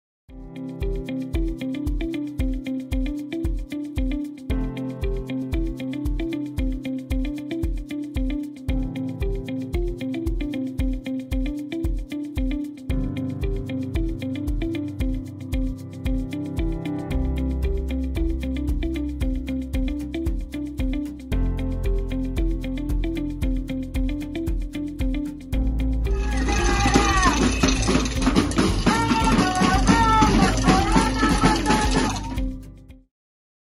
Colonia el Polvorín, Jesús Corona.
El candidato a la presidencia municipal de Cuautla por los partidos PAN-PHM, Jesús Corona Damián, realizó recorrido en la colonia el Polvorín.